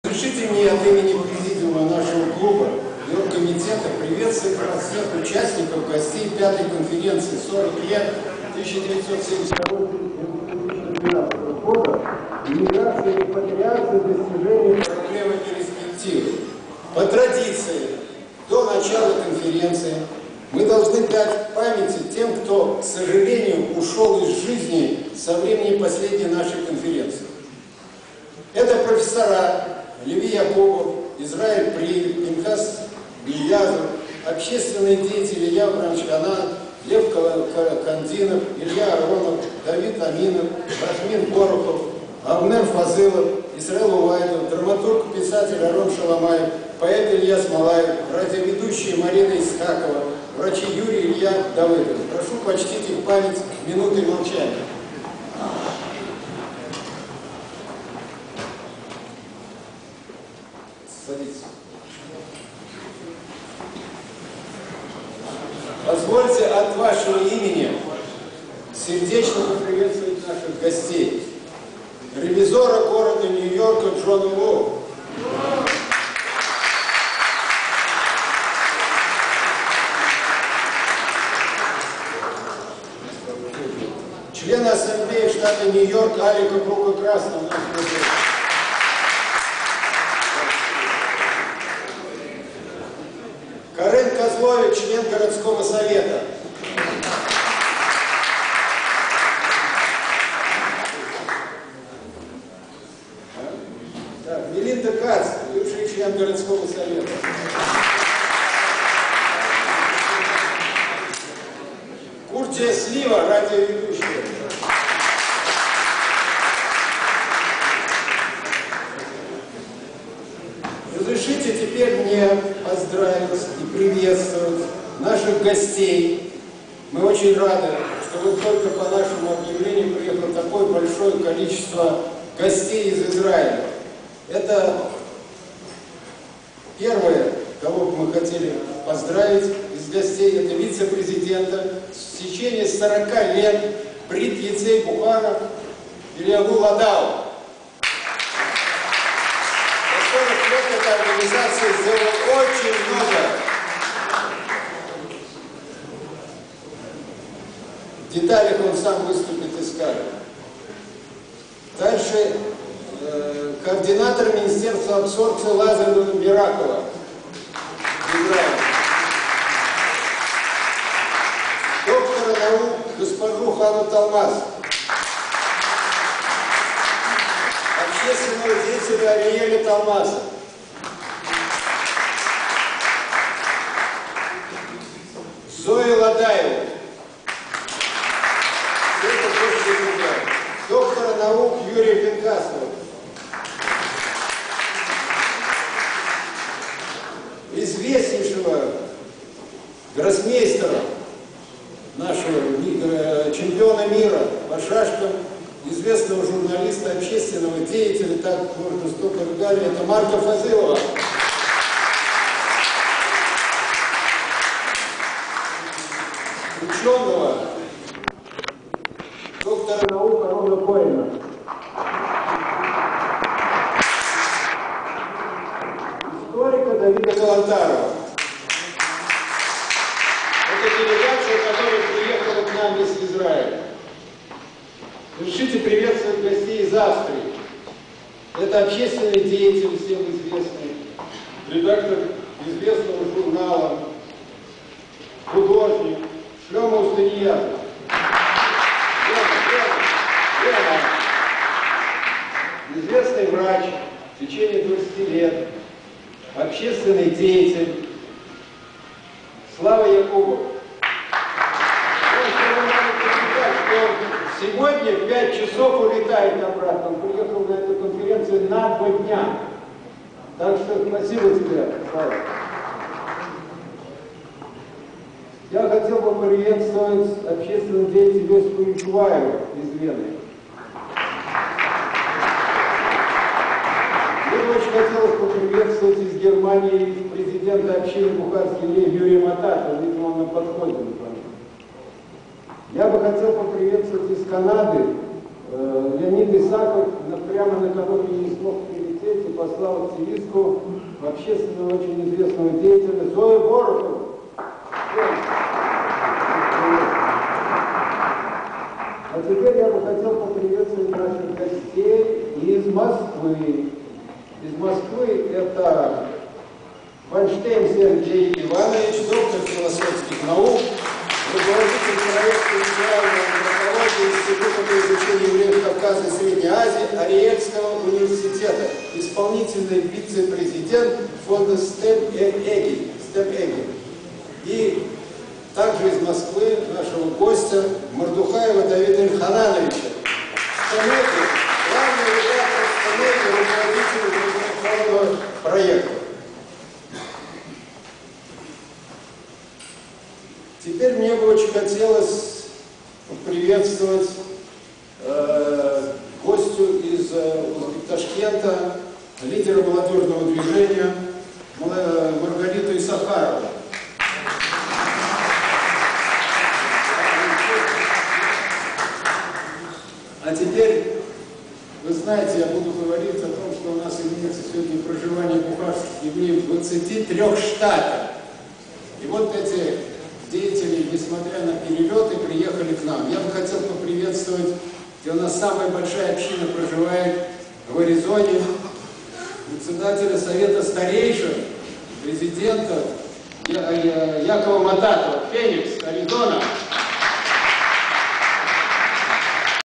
001.MPG, Misha Normatov, Dr. ROBERT PINKHASOV
5st   CONFERENCE 40 YEARS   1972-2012   IMMIGRATION AND REPATRIATION OF THE BUKHARIAN JEWS In BUKHARIAN CENTER-SYNAGOG.   10/21/2012  NEW YORK